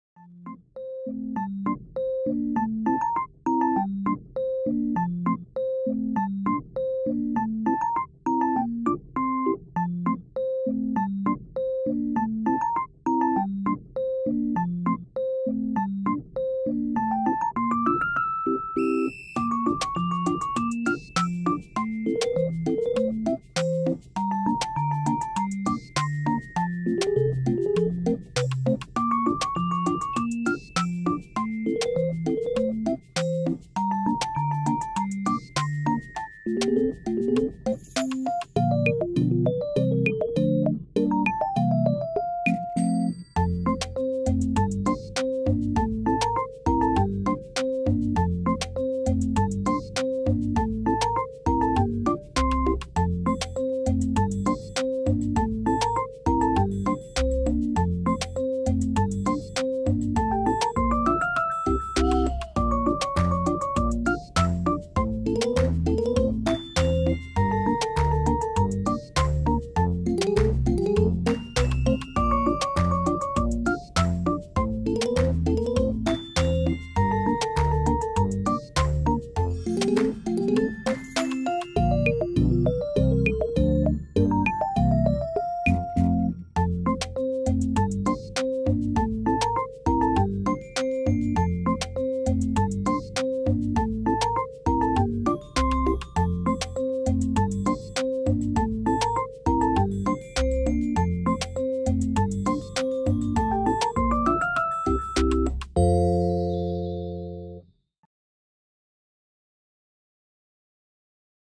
Drawing Tighnari Chibi Version - Genshin Impact | Desenhando Tighnari Versão Chibi | Speedpaint
Oioi! Postando esse vídeo que tava guardado há um tempinho, pro canal não ficar muito tempo sem atualização.